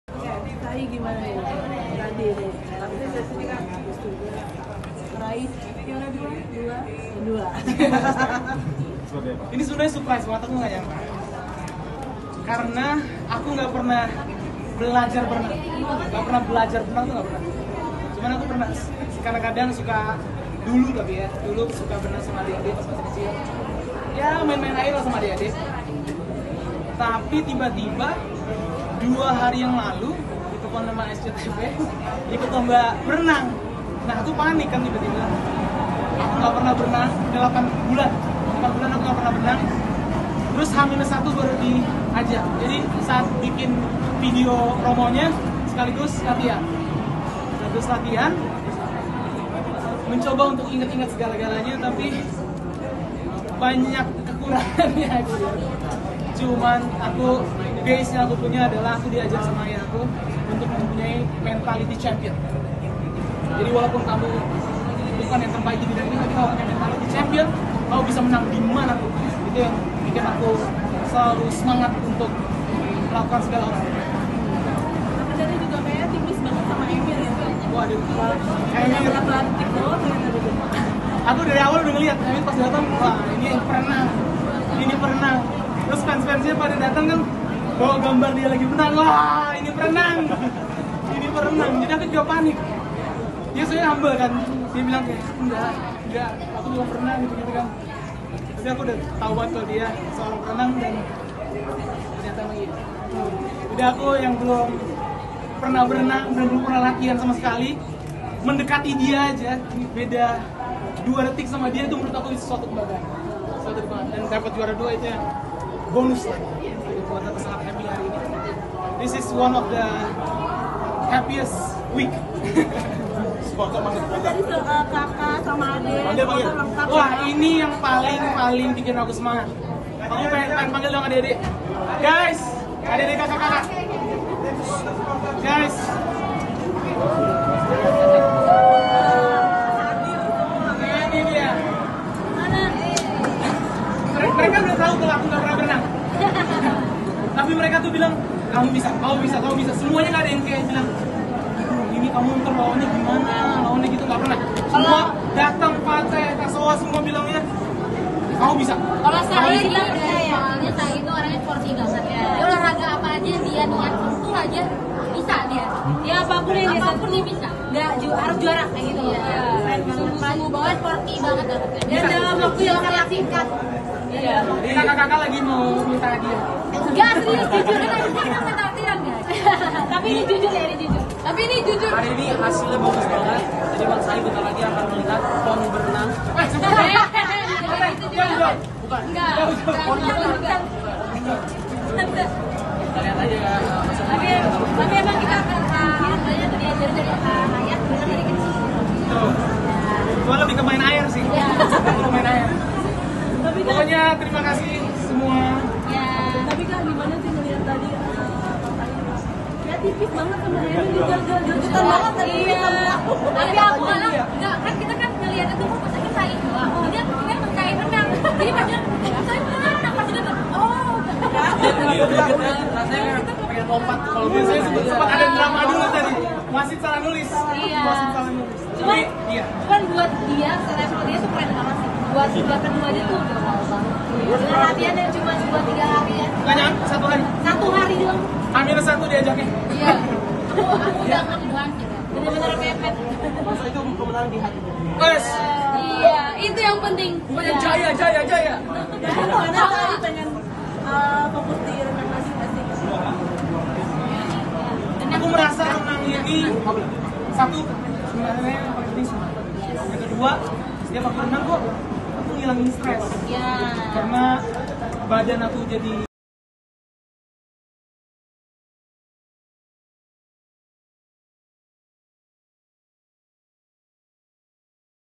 Juara 2 Lomba Renang, Saaih Halilintar Ungkap Punya Mental Pemenang Didikan Sang Ayah
Saaih Halilintar berhasil menjadi juara dua dalam perlombaan renang pria 50 meter Turnamen Olahraga Selebriti Indonesia (TOSI) Season 3 yang digagas SCTV dan RANS Entertainment.

Adik Atta Halilintar tersebut mengaku bersyukur bisa keluar sebagai juara meskipun dengan latihan yang sangat minim. Saaih diajak ikut lomba satu hari sebelum TOSI digelar.